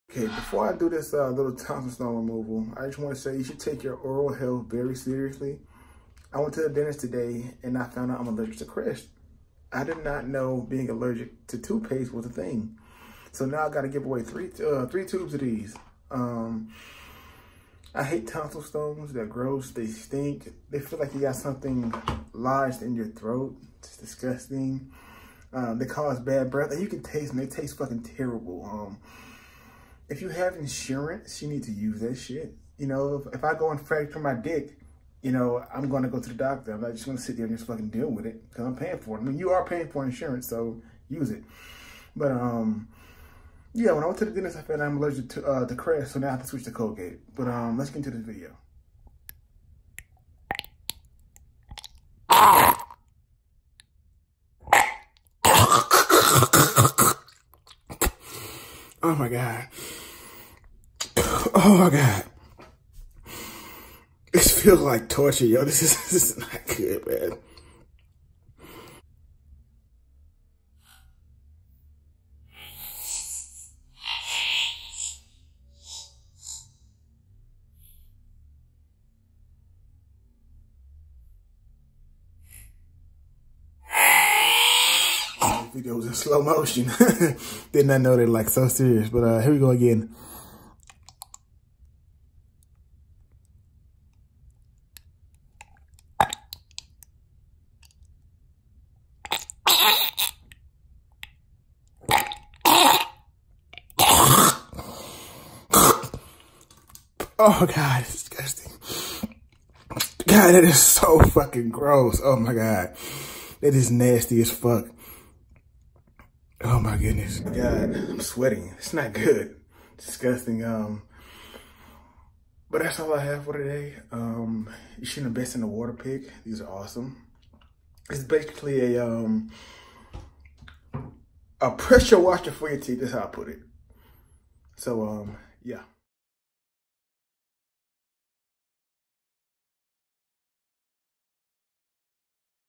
DIY Tonsil Stone Removal
I hate tonsil stones. Theyre annoying as hell & irritating. I DON’T enjoy doing this!